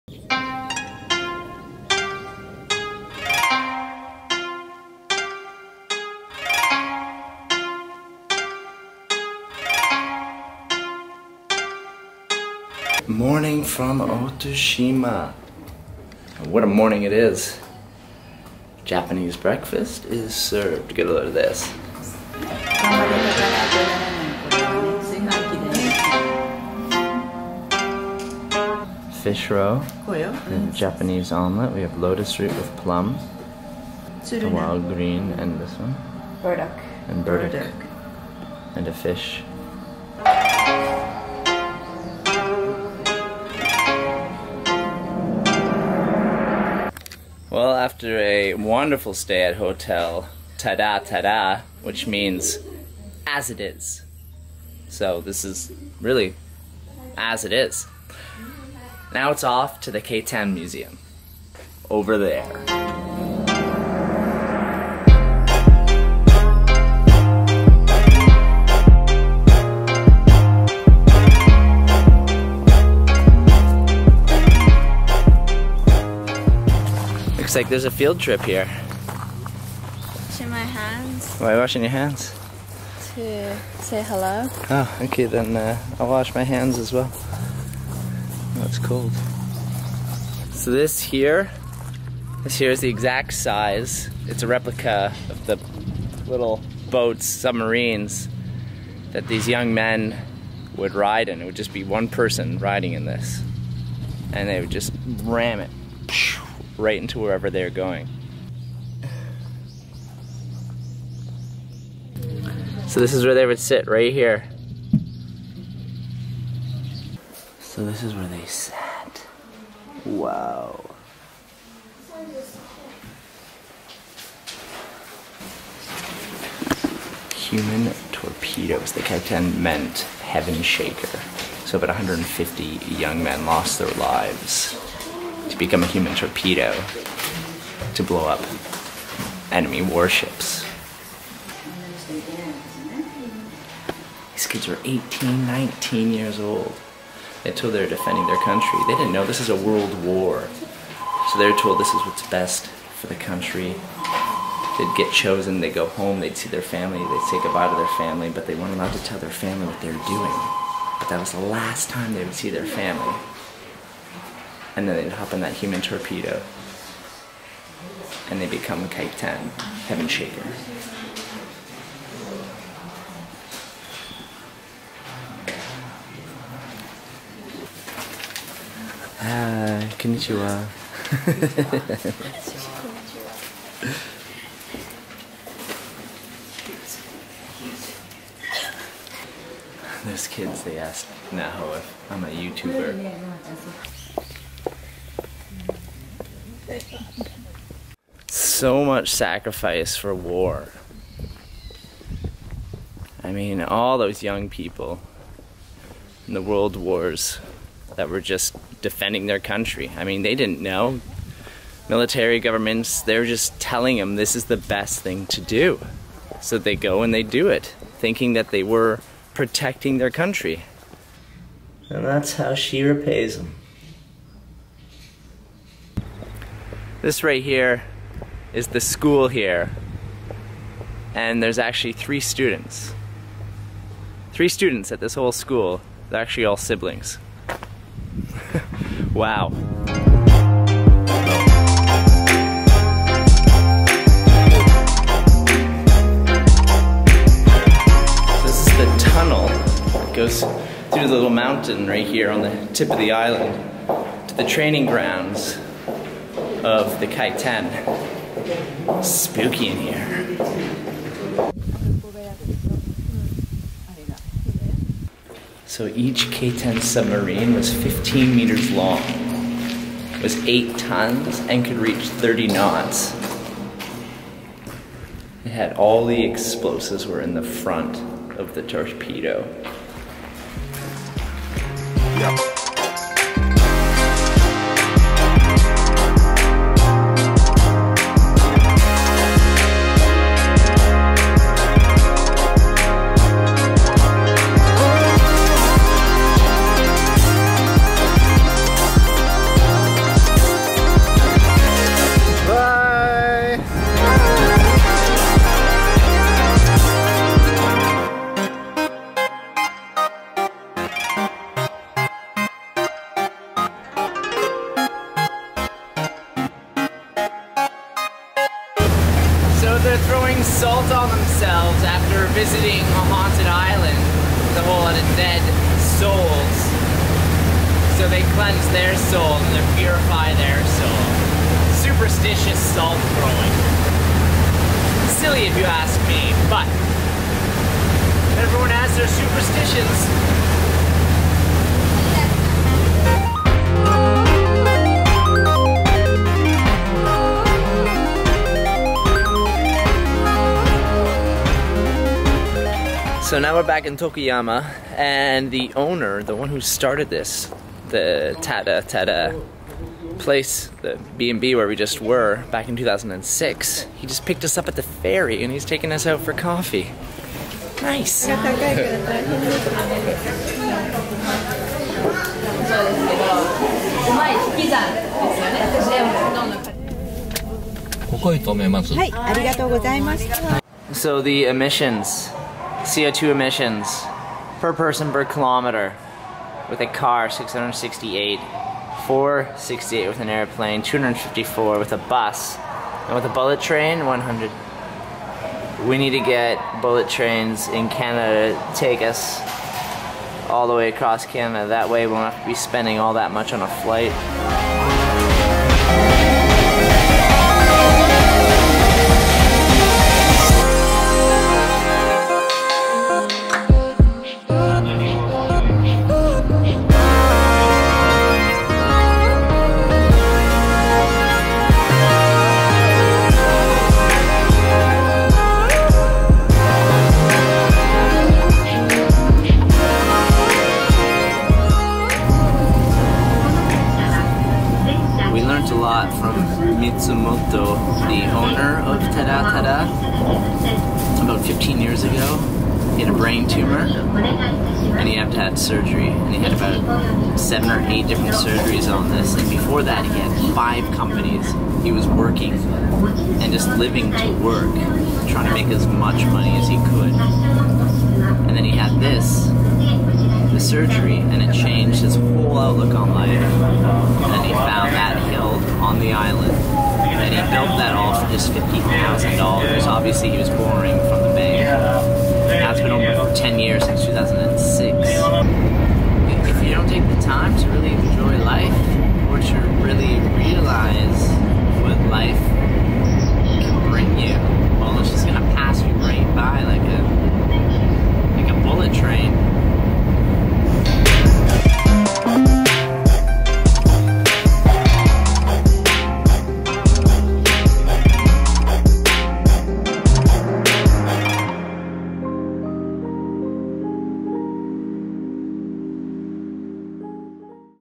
Vlog 55 - They were human torpedoes
Visited the Kaiten Museum. They were the last resort to fight World War II for the Japanese. They were literally human torpedoes. Suicide bombers.